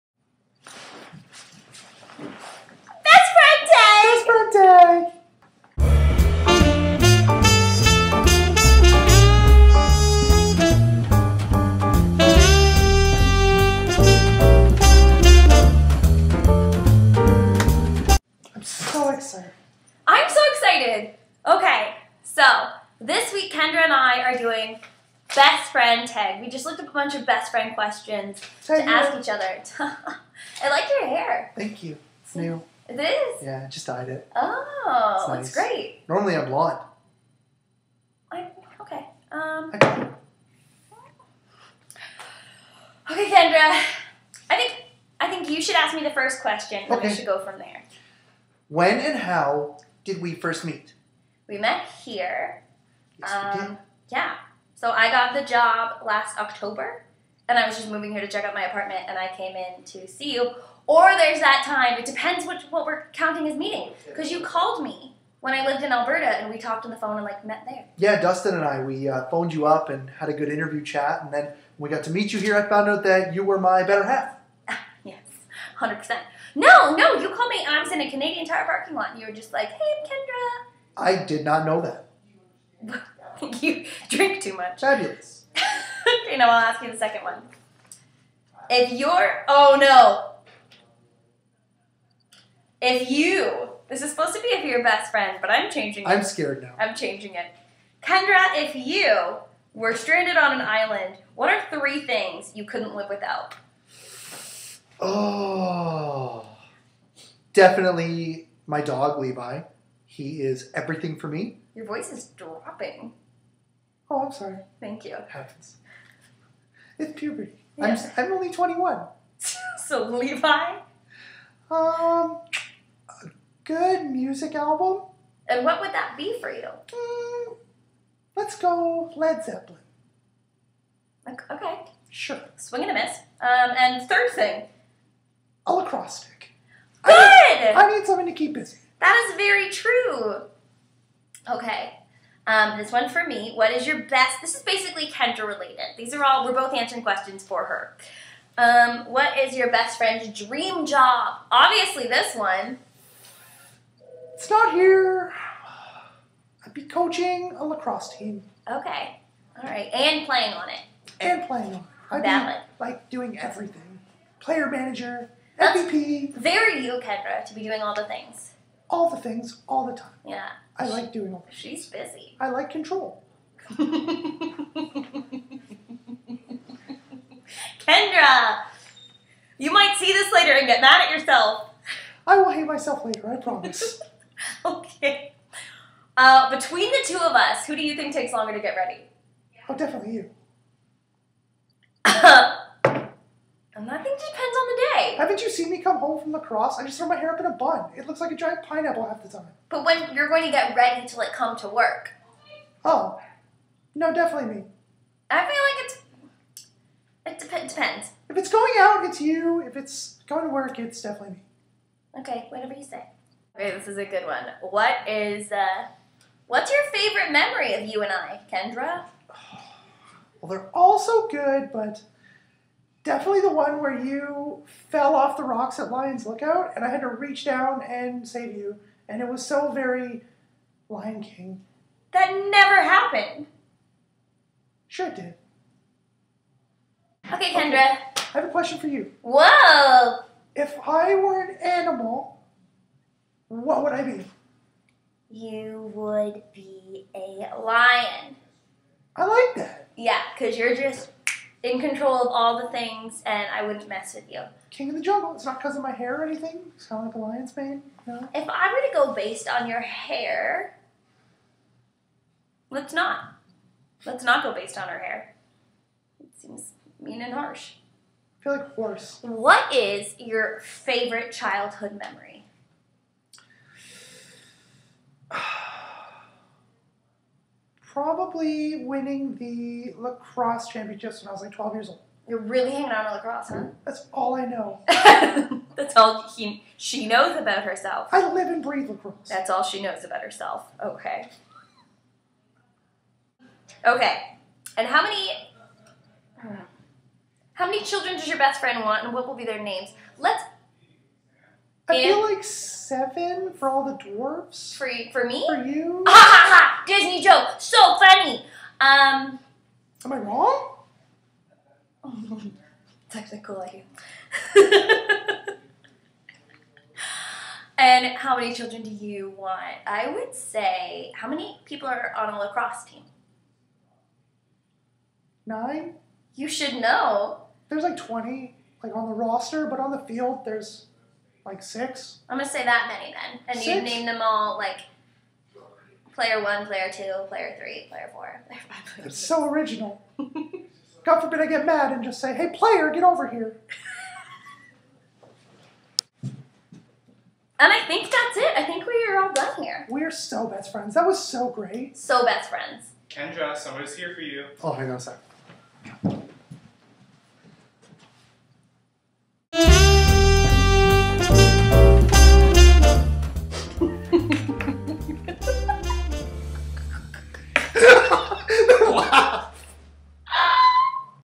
Coffee Break with Olivia and Kendra: Best Friend Tag!
Olivia and Kendra test their best friendship with a series of questions.